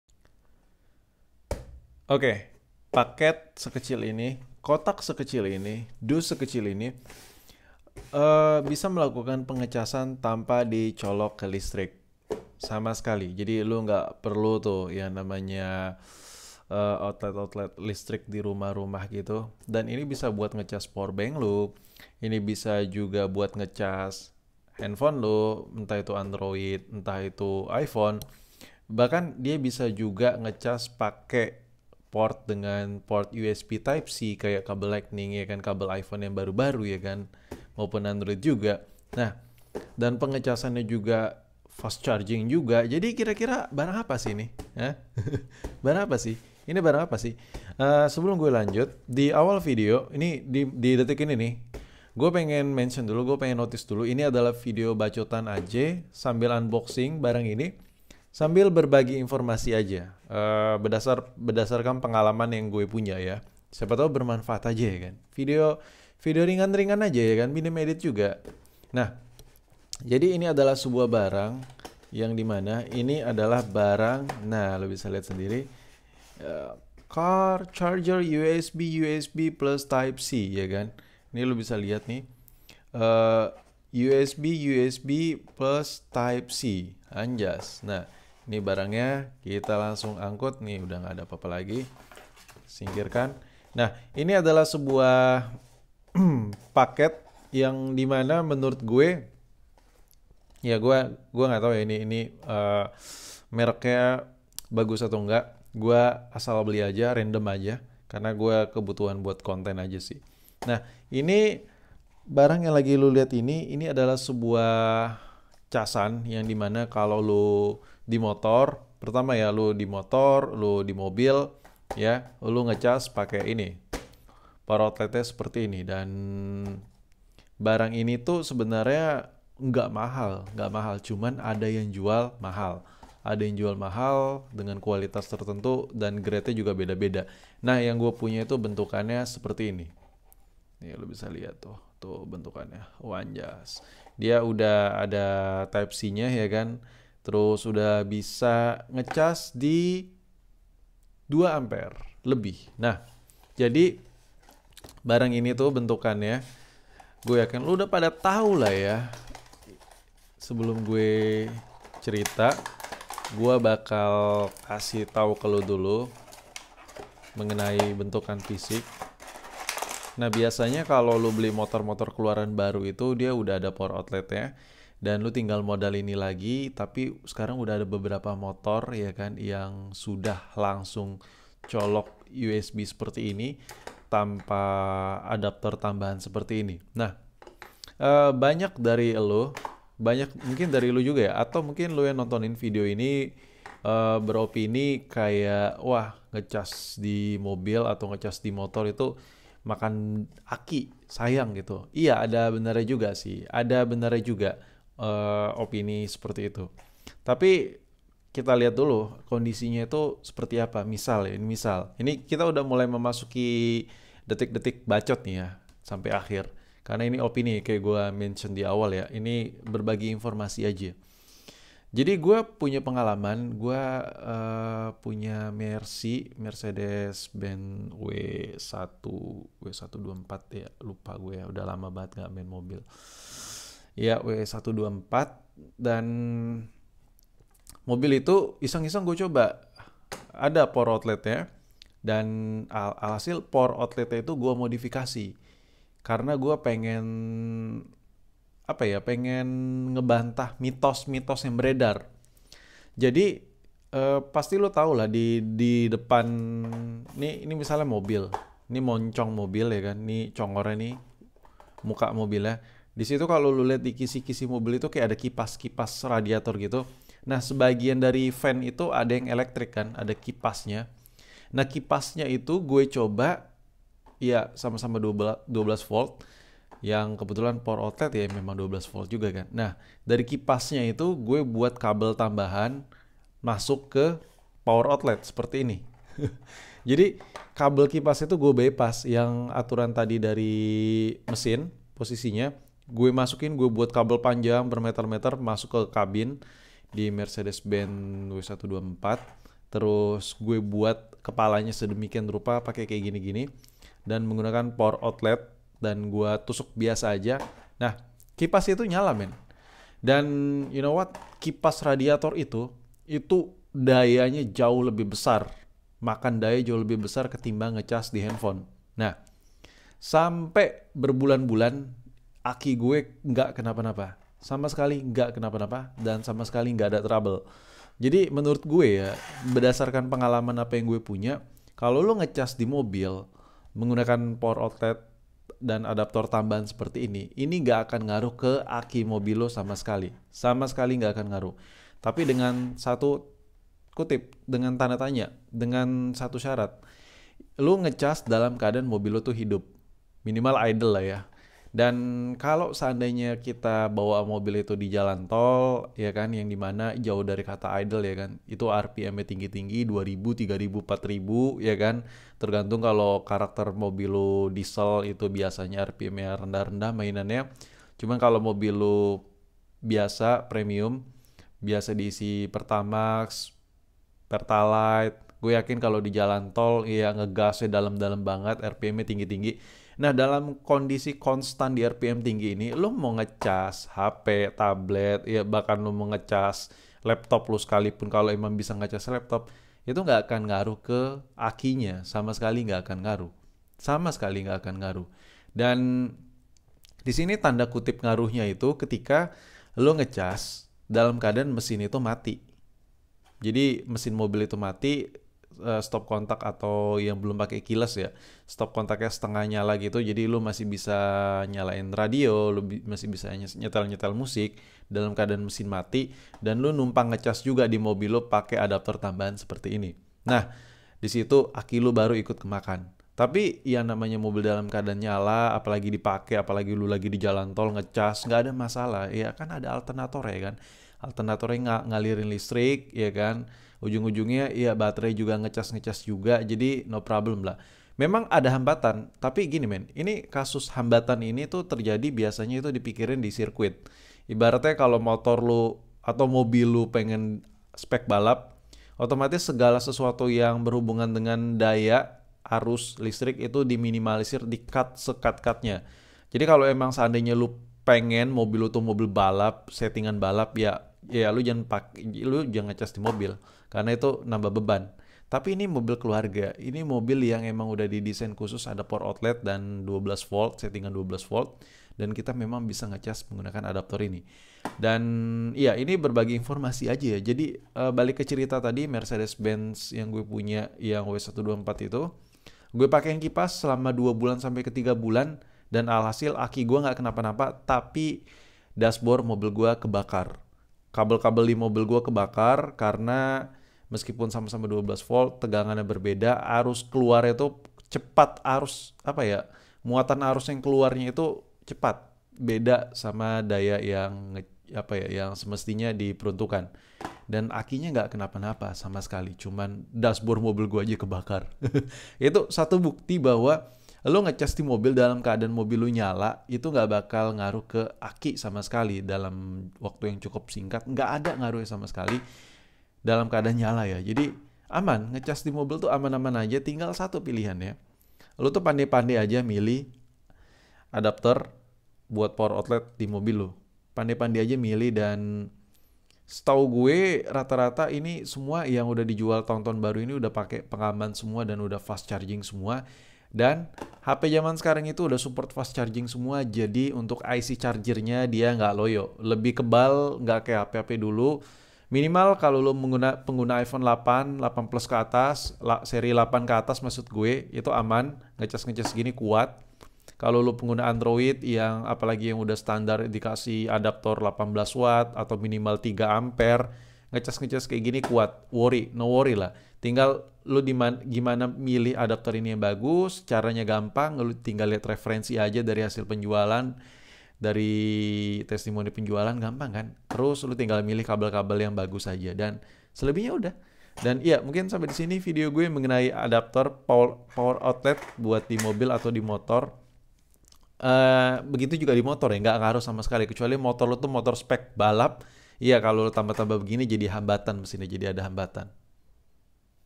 Ngecas di Mobil aman Gak?? Buat yang belum tau ini jawabannya
Pada dasarnya hp jaman sekarang ic nya udah pada badak badak semua, malah udah ada yang ngecas sampai ke 60Watt an.

Jadi Ngecas di mobil itu aman aman saja, selagi kabel dalam Kondisi baik. Bahkan jauh lebih safety ketimbang power outlet di rumah.
Karena dashboard mobil lebih unggul di fuse, sedangkan power outlet/colokan di rumah pengaman nya lebih ke si adaptor itu sendiri jika terjadi short arus, mangkanya ada saja kasus orang tersetrum saat ngecas, karena sekring rumahan itu tahanan nya harus Besar untuk nge cut arus pendek, kalau sekelas koslet ic gak bakal kuat buat mutusin arus di sekring rumah, kec arus pendeng langsung.